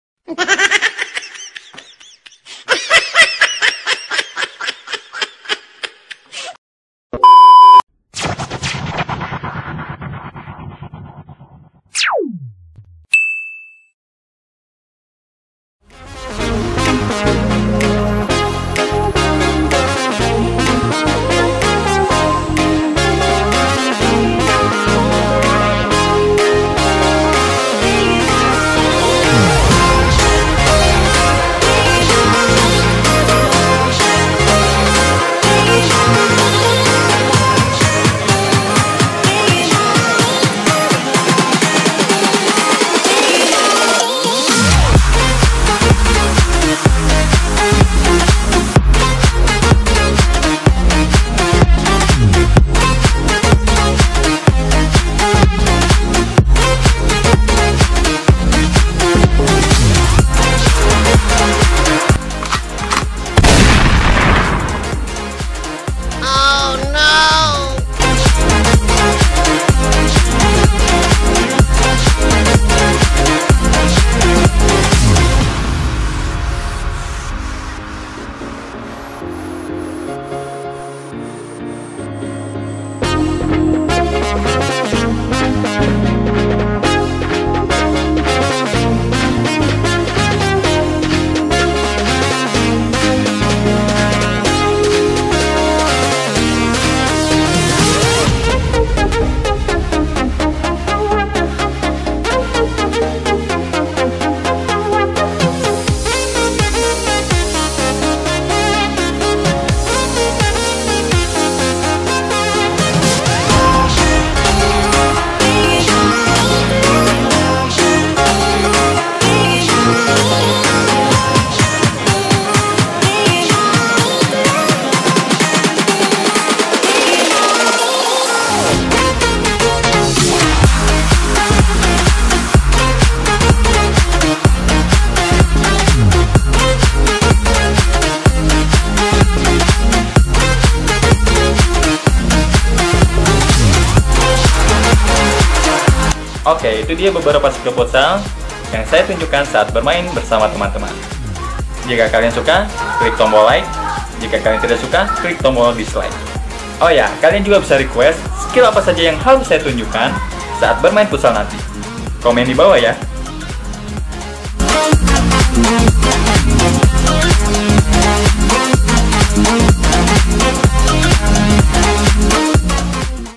MIRIP RICARDINHO? Futsal Skills & Tricks | Ady Rikudo
HALO GUYS
Ini merupakan kumpulan video latihan futsal saya bareng teman-teman yang ke sekian kalinya di upload di youtube. Tapi sekarang kualitasnya lumayan bagus, gak kayak yang dulu" :D

Disini saya memakai jersey futsal Inter Movistar, dimana salah satu idola saya yaitu Ricardinho main di Tim ini. Btw, skillnya mirip gak? wkwkwk

Selamat menonton, semoga bisa menghibur :D

Skill Futsal Indonesia
Skill Futsal Ricardinho